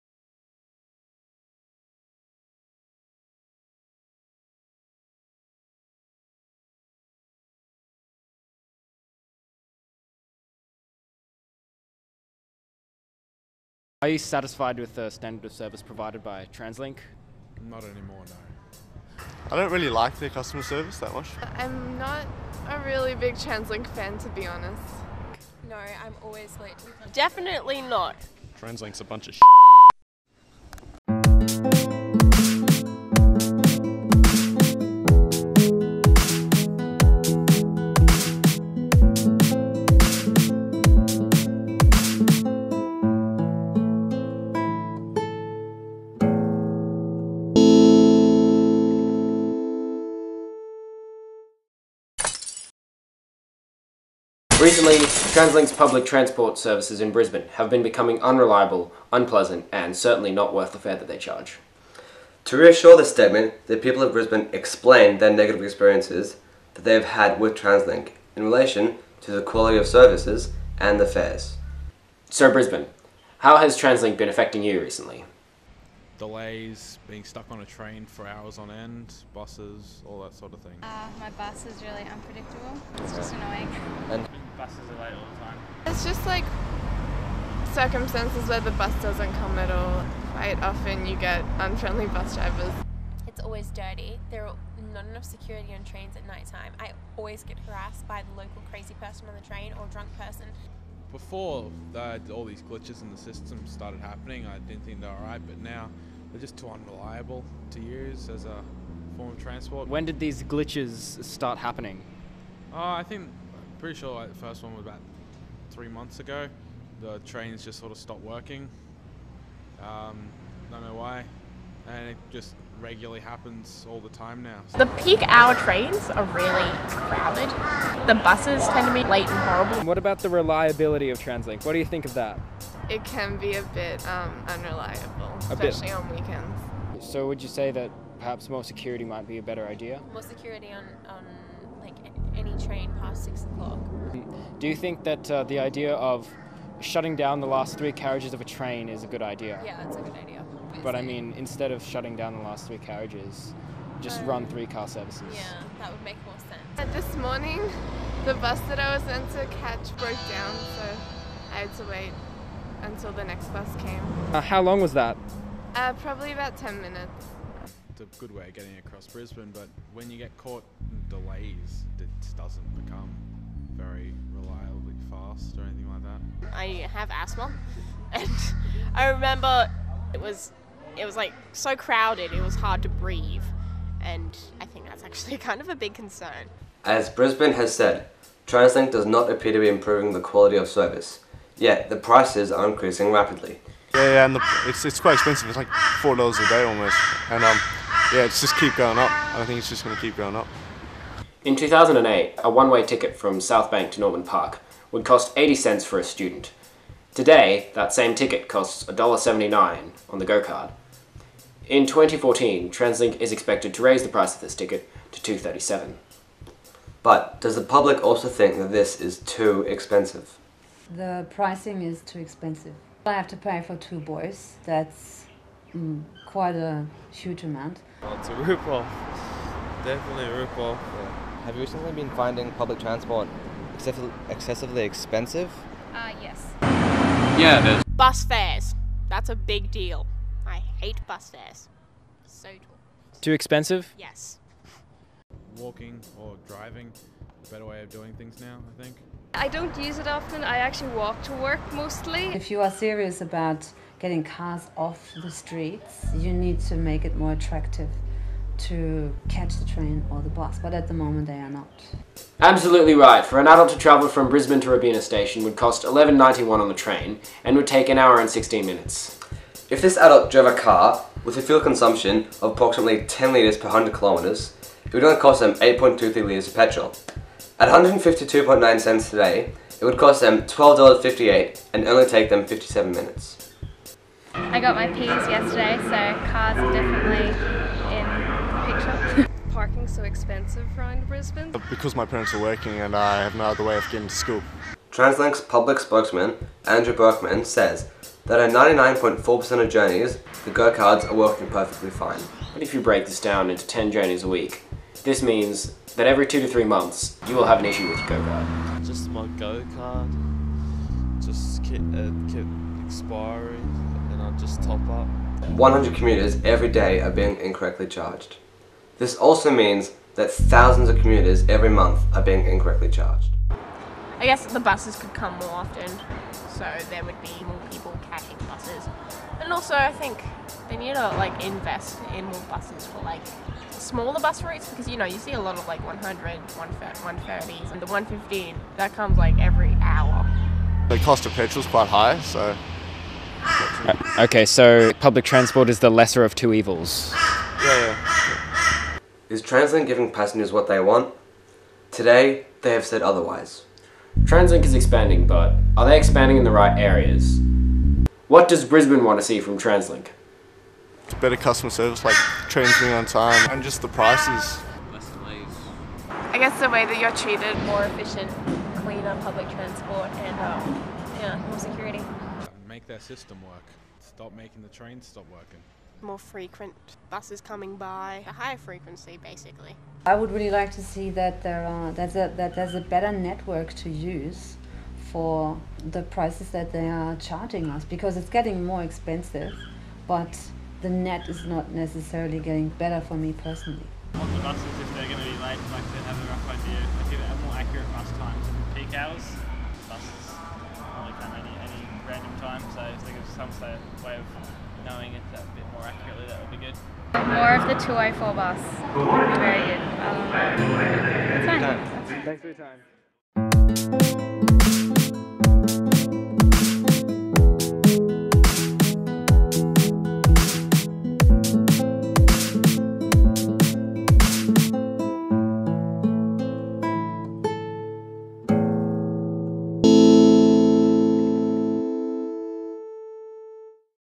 The Broken Link
The Broken Link will explore how Translink charges too much given the quality of service provided and how this deters people from catching public transport. This documentary aims to enlighten illogical decisions made by transport ministers.

The task was to construct a documentary on a current issue, and present it in such a method that will reach the intended audience and will therefore instigate change.

- We were unable to obtain permission from Brisbane Transport or Queensland Rail staff for an interview.
- The audio is sporadic at points because there were some peoples' voices who were so quiet compared to others. We even lifted the volume by 21 decibels and some were STILL so quiet!
- Our Film & TV teacher advised us against using a tripod because it would be too confronting for interviewees, so at parts it does tend to be shaky
- We are just year 12 students, not professionals, so we don't exactly have cinema-quality equipment.

Don't forget to give us feedback on how well or bad we did, so that we know what to do next time!